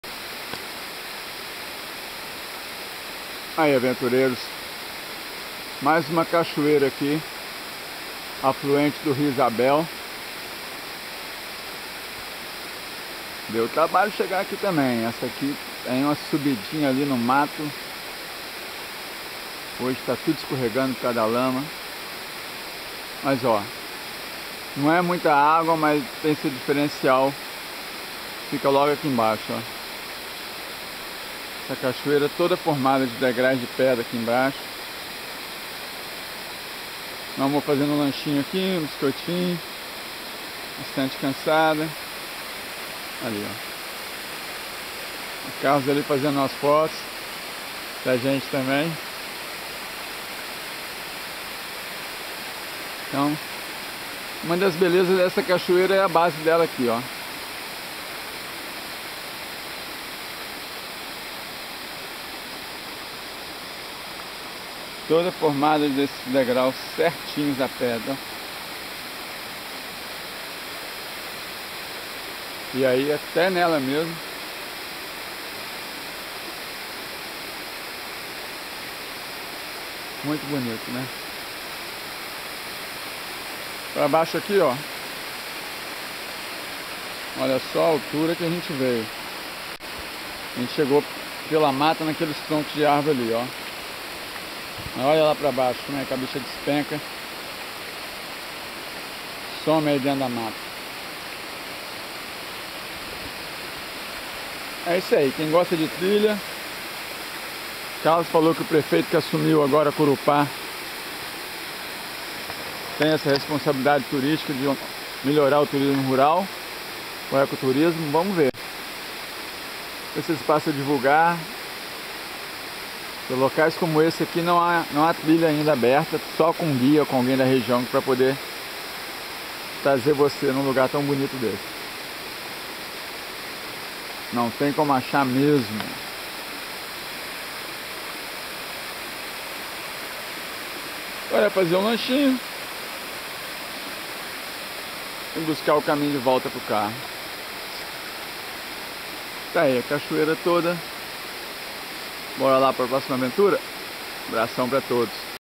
Cachoeira dos DEGRAUS - Ep. 123
05/04/2017  - Corupá, SC, após a cachoeira alta, continuamos na mata por outra trilha ate esta Cachoeira dos Degraus, valeu o esforço. Carlos nos presenteou com muitos produtos orgânicos, e de despensa cheia partimos para Corupá, para conhecer o Seminário, onde nos permitiram pernoitar, e dia 6/04 descemos para Floripa, passando por Bombinhas, Palmas e outras praias.